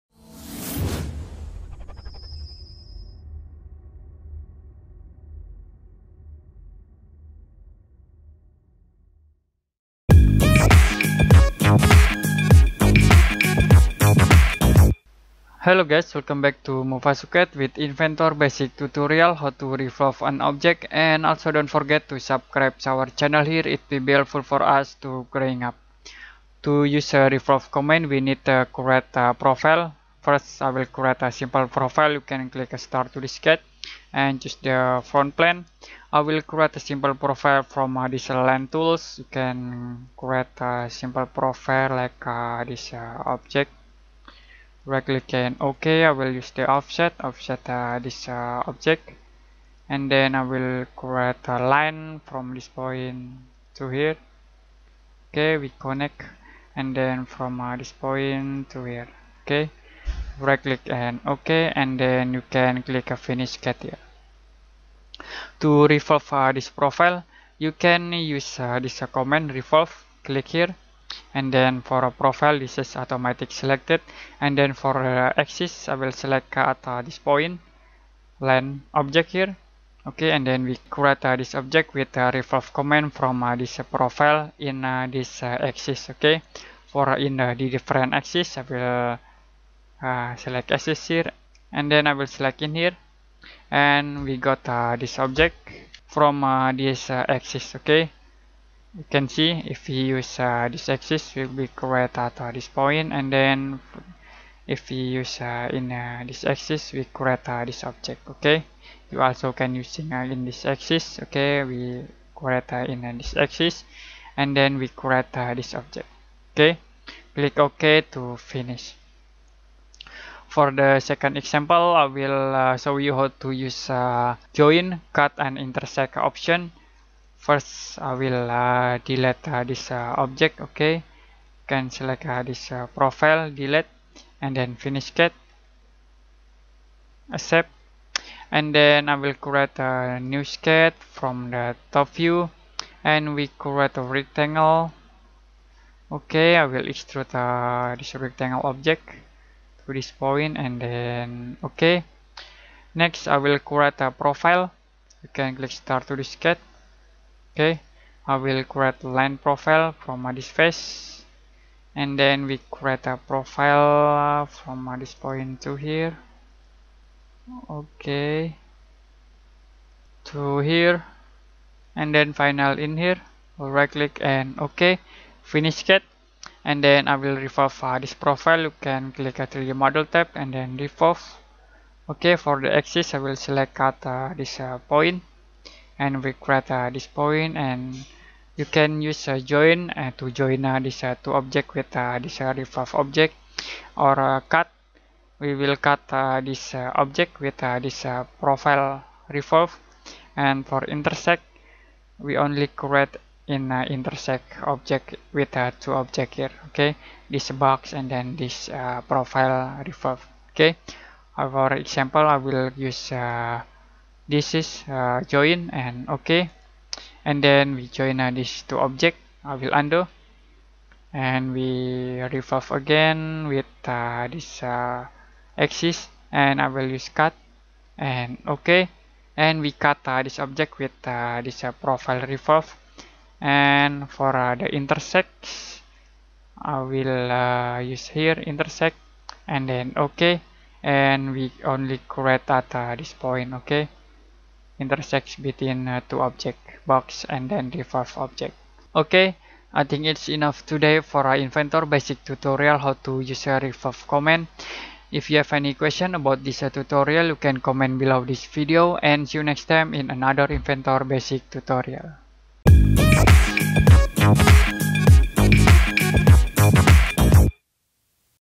Inventor How To Revolve
Autodesk Inventor training and course tutorial how to revolve an object for beginner at basic level with some easy steps, check it out!!!